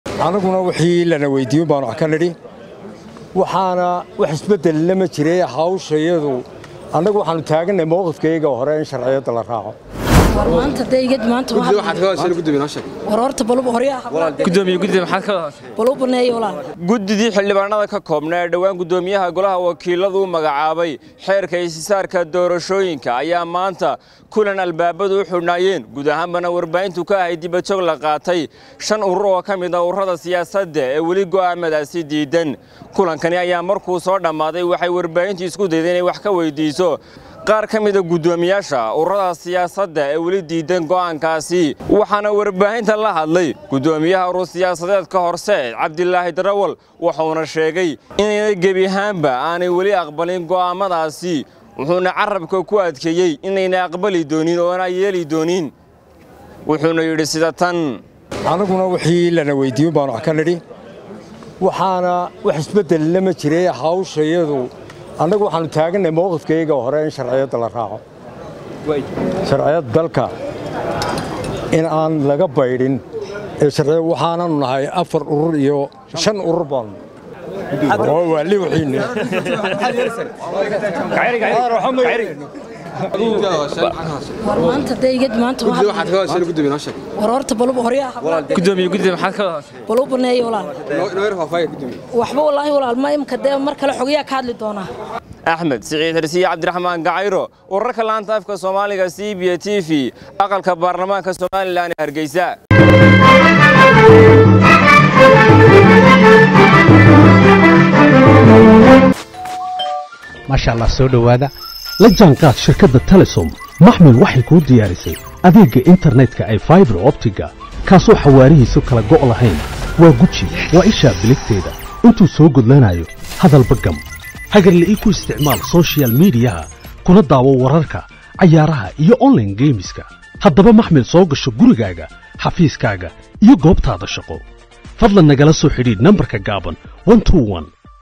“Mawqifkayagii hore uun ayaanu ku taaganahay. ” Guddoomiye Cabdilaahi Darawal
“Mawqifkayagii hore uun ayaanu ku taaganahay. Haddii dhibaatooyin kale ina haystaan inaan dastuurkii baabiino ma aha” Guddoomiye  Cabdilaahi Darawal.